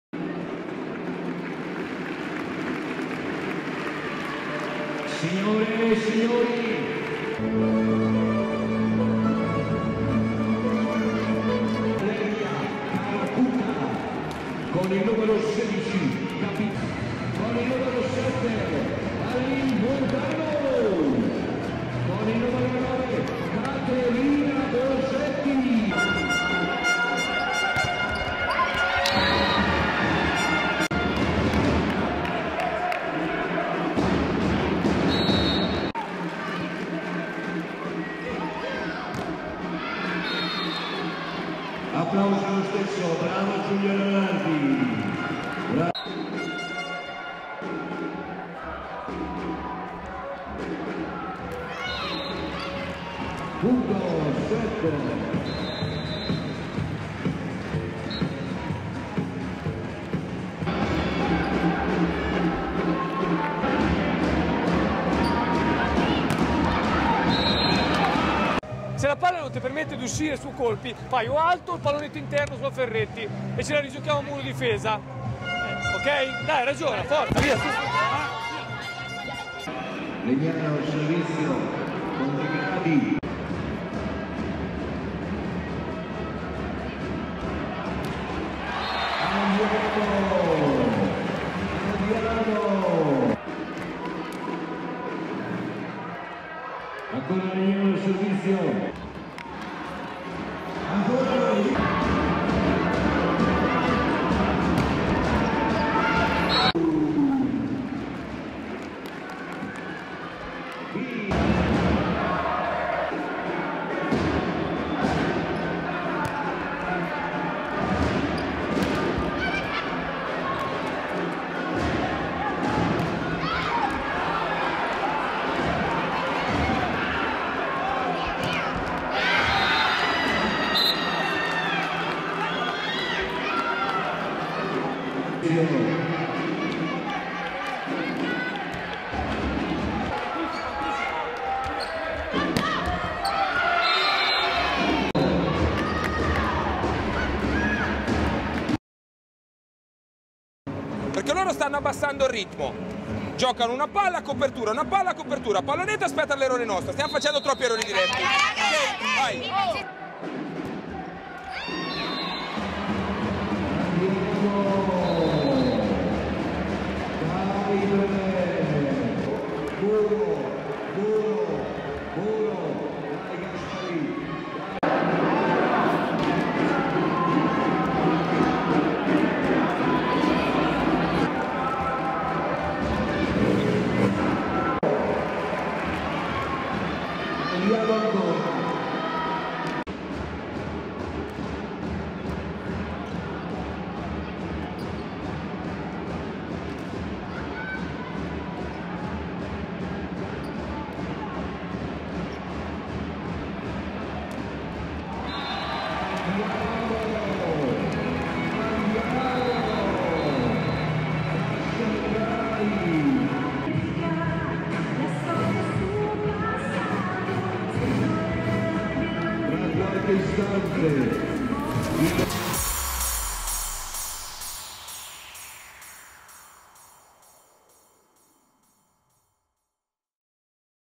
Modena - Legnano | Highlights | 7^ Giornata | Samsung Galaxy Volley Cup 2017/18
Gli highlights del match della 7^ giornata della Samsung Galaxy Volley Cup 2017/18 tra LiuJo Nordmeccanica Modena e SAB Volley Legnano